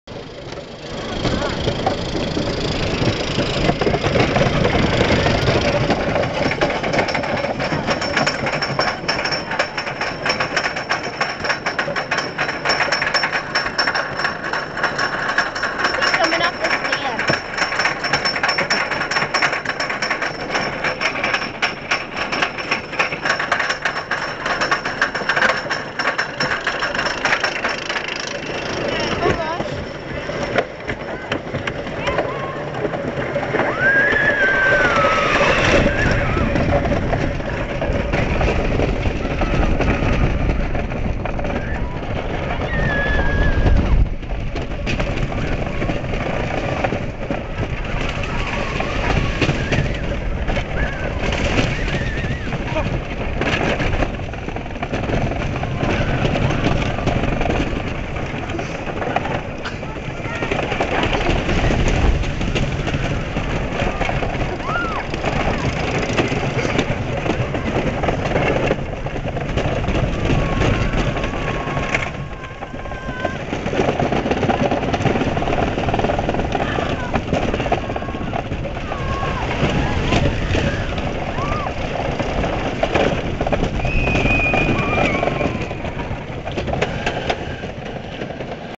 Funtown Splashtown "Excalibur" Roller coaster Video From Seat
Video taken from the front seat of the excalibur rollercoaster at Funtown Splashtown.